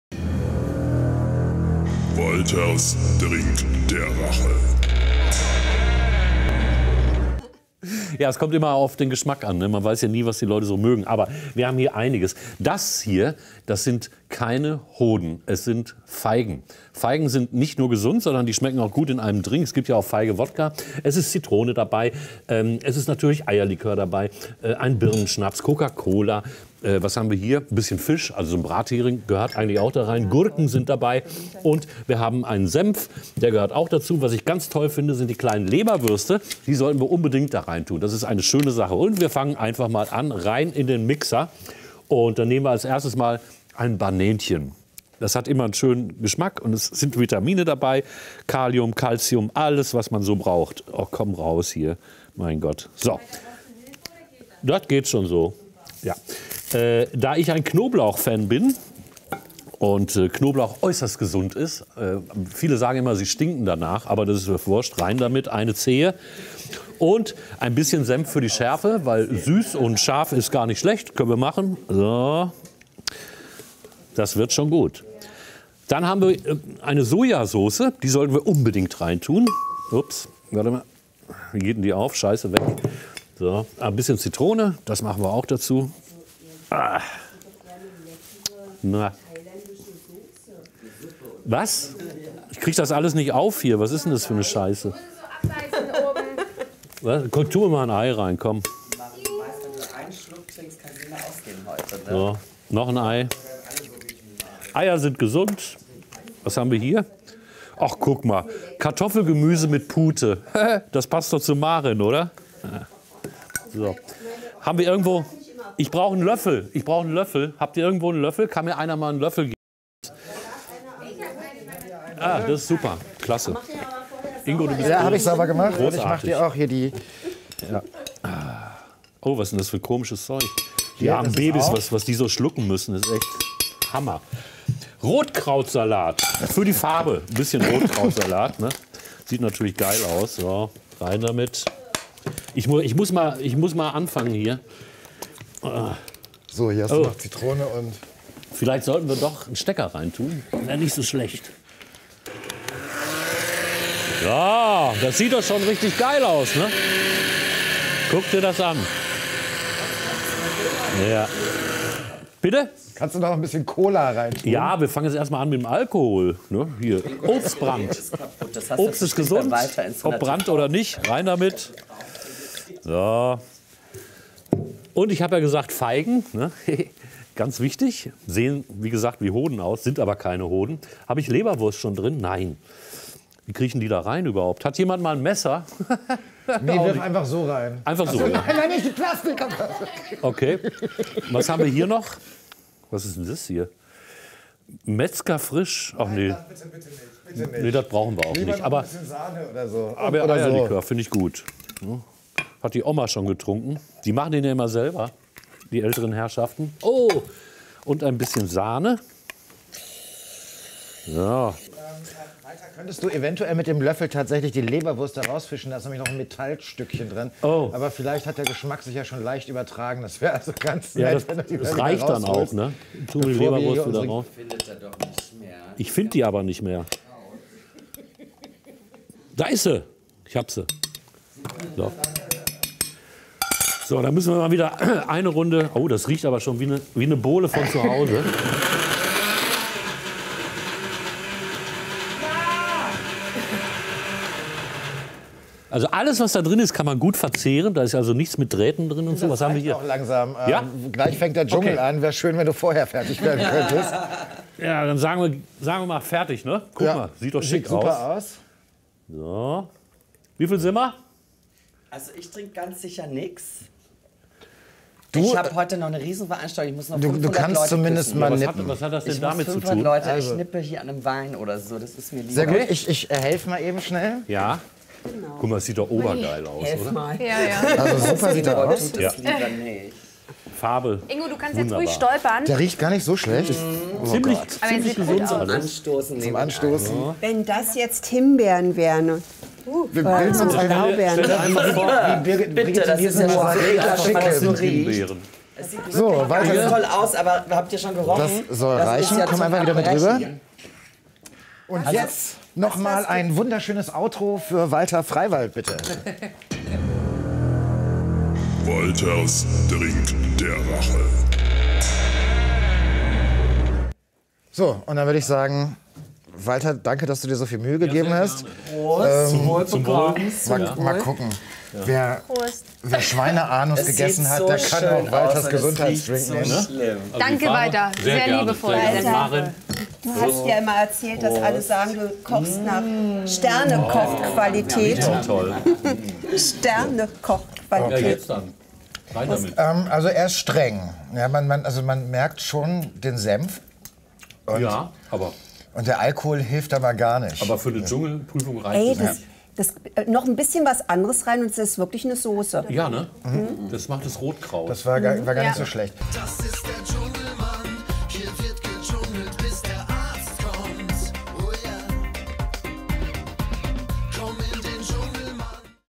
Walter Freiwalds Drink der Rache - Prost!
Walter Freiwald schau tief ins Glas und präsentiert uns seinen "Drink der Rache".